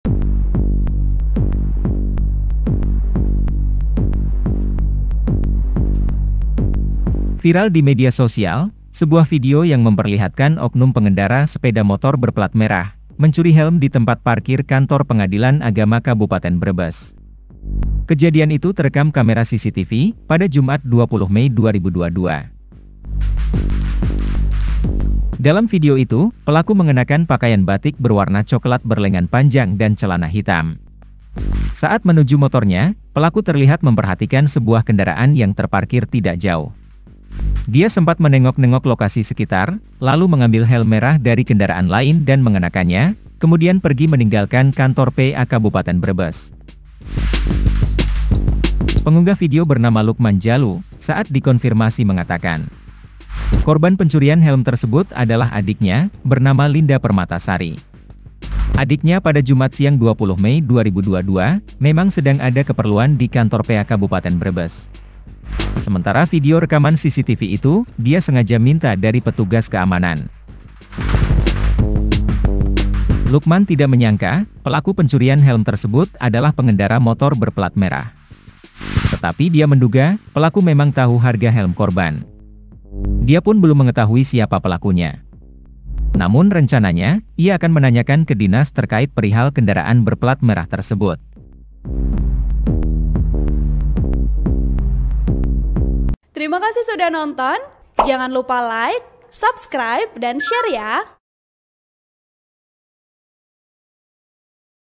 PEMOTOR PLAT MERAH CURI HELM di Kantor Pengadilan Agama
Kejadian itu terekam kamera CCTV pada Jumat (20/5/2022).  Satu di antaranya diunggah oleh akun Lukman Jalu di grup Pemali Brebes.

Dalam video itu, pelaku mengenakan pakaian batik berwarna cokelat berlengan panjang dan celana hitam.

Saat menuju motornya, pelaku terlihat memperhatikan sebuah kendaraan yang terparkir tidak jauh. Dia sempat menengok-nengok lokasi sekitar.

Setelah itu, mengambil helm merah dari kendaraan lain dan mengenakannya. Dia kemudian pergi meninggalkan Kantor PA Kabupaten Brebes.

Penulis: Fajar Bahruddin Achmad | Editor: deni setiawan